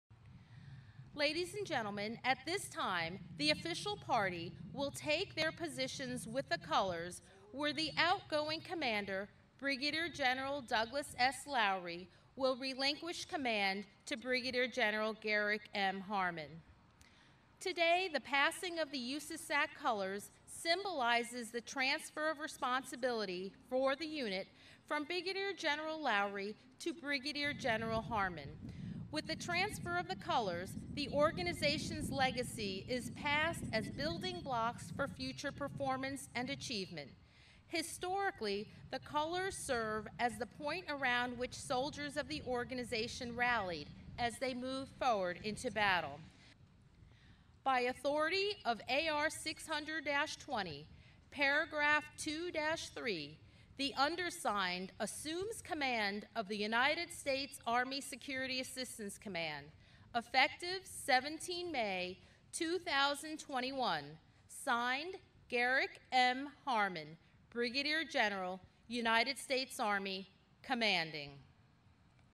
USASAC Change of Command
AL, UNITED STATES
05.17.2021

B-Roll of the USASAC Change of Command. BG Garrick Harmon takes command of the United States Army Security Assistance Command, as outgoing commander BG Doug Lowrey becomes commanding general of the Mission and Installation Contracting Command headquartered at Joint Base San Antonio/Fort Sam Houston.

Film Credits: Video by Tim Hanson
U.S. Army Security Assistance Command

Tim Hanson
change of command
USASAC
Army Security Assistance Command
Brig. Gen. Douglas Lowr
Brig. Gen. Garrick Harmon